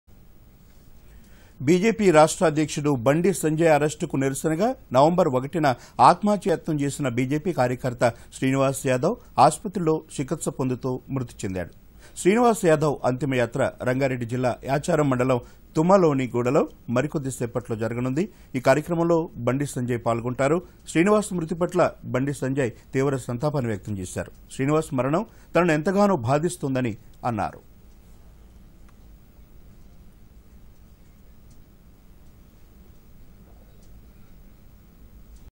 BJP activist Srinivas, who committed suicide on November 1st died.
బీజేపీ రాష్ట్ర అధ్యక్షుడు బండి సంజయ్‌ అరెస్ట్‌కు నిరసనగా నవంబర్‌1న ఆత్మహత్యాయత్నం చేసిన బీజేపీ కార్యకర్త శ్రీనివాస్‌ ఆస్పత్రిలో చికిత్స పొందుతూ మృతి చెందాడు.  శ్రీనివాస్‌యాదవ్ అంతిమయాత్ర రంగారెడ్డి జిల్లా యాచారం మండలం తుమ్మలోని గూడలో మరికొద్ది చేసట్లో  జరగనుంది. ఈ కార్యక్రమంలో బండి సంజయ్‌ పాల్గొననున్నారు. శ్రీనివాస్ మ్రుతి పట్ల బండి సంజయ్ తీవ్ర సంతాపం వ్యక్తం చేశారు.శ్రీనివాస్ మరణం తనను ఎంతగానో బాధిస్తోందన్నారు.